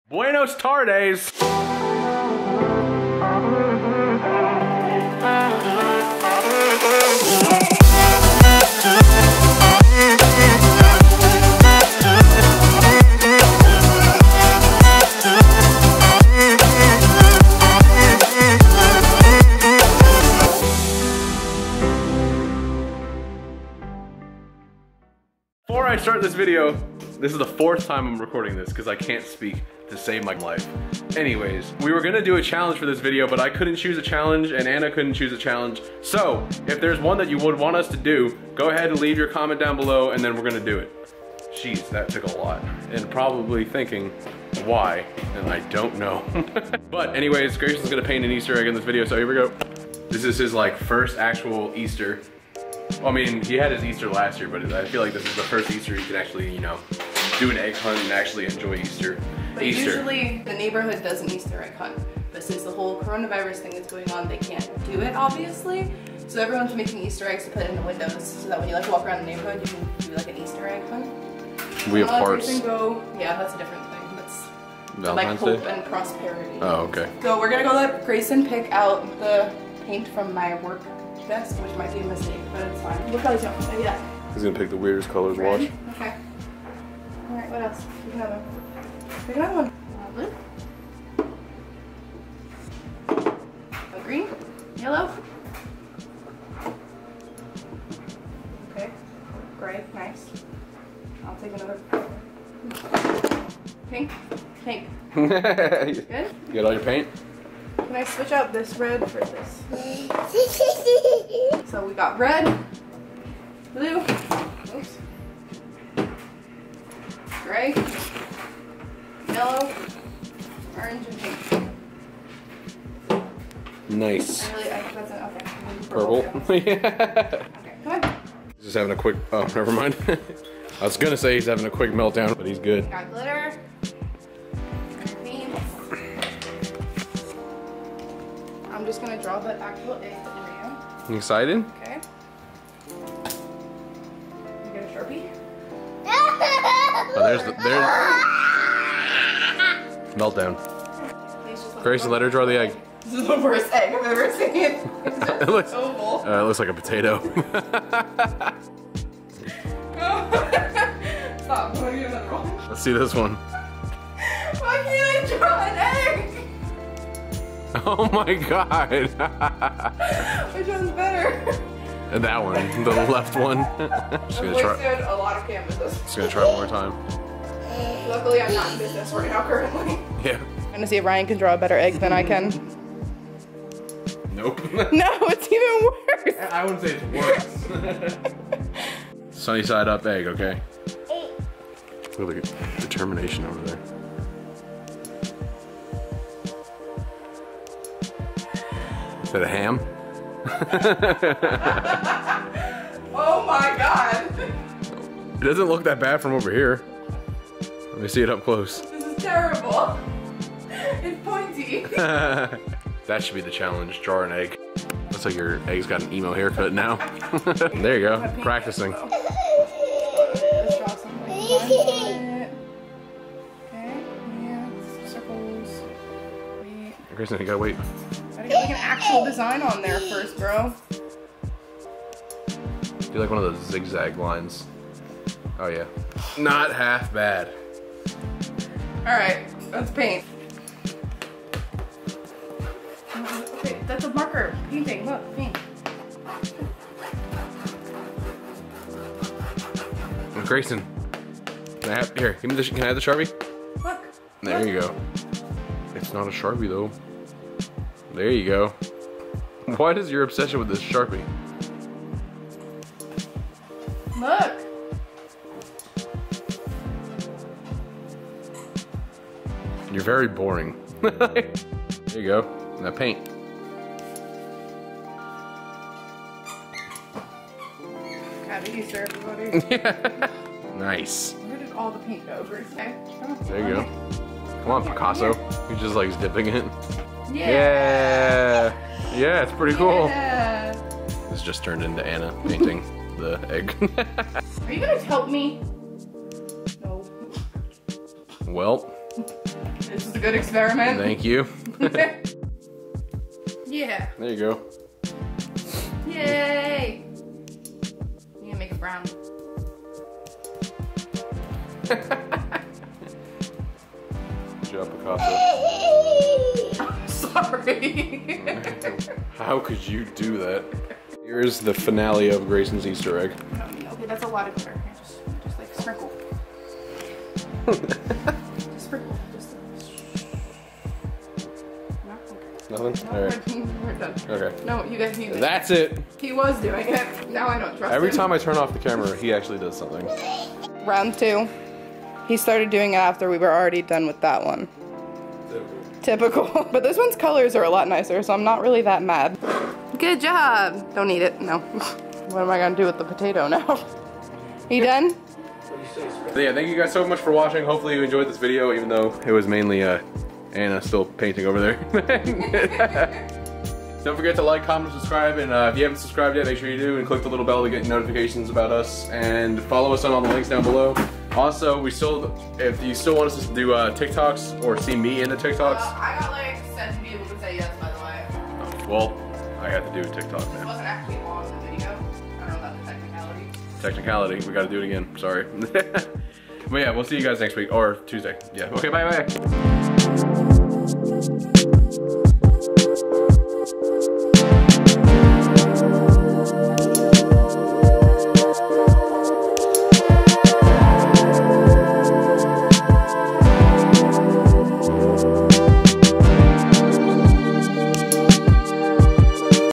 Toddler paints Easter eggs!
Hey everyone! Sorry this upload is late, it took FOREVER to upload it. Thank you for your patience and enjoy the video! Greysen tries to paint his own Easter eggs! Anna also struggles to draw an egg. Its actually harder than it looks haha. We hope you have a great day, and a great Easter. If you can't enjoy it with family due to the virus, we hope you find a way to make it special! Thanks everyone, we'll see you on Tuesday!

Free entertainment is cool right? New videos are on